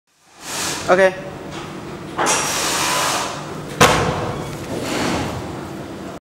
Vibration Test for Pinwheel hoverboard smart balance wheel balance scooter
Violence test - Vibration Test. Test whether pinwheel hoverboard can keep working in virbration condition.